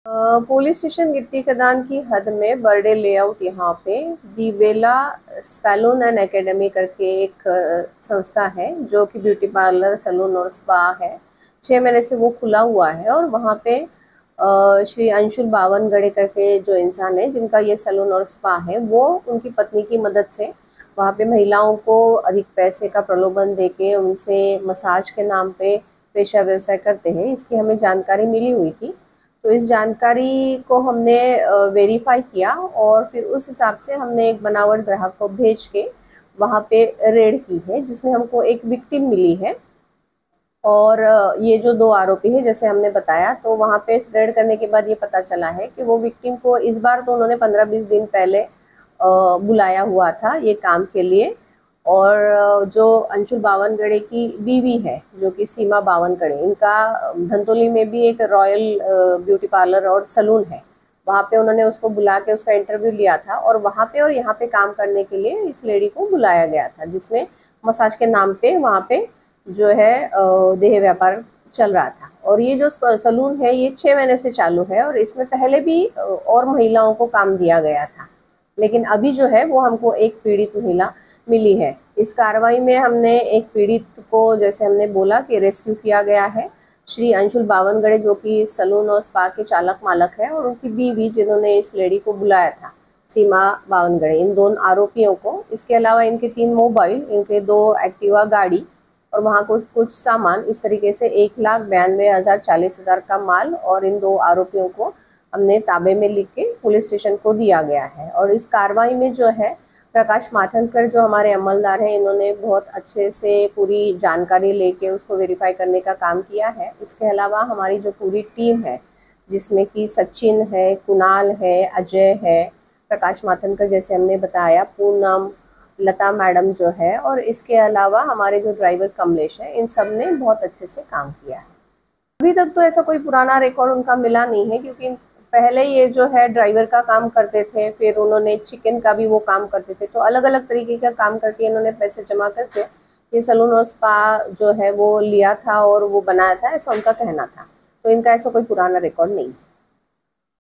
स्पा और मसाज के नाम पर देहव्यापार गिट्टीखदान पुलिस स्टेशन अंतर्गत मामला आया सामने
गिट्टीखदान पुलिस स्टेशन के अंतर्गत स्पा और मसाज के नाम पर देहव्यापार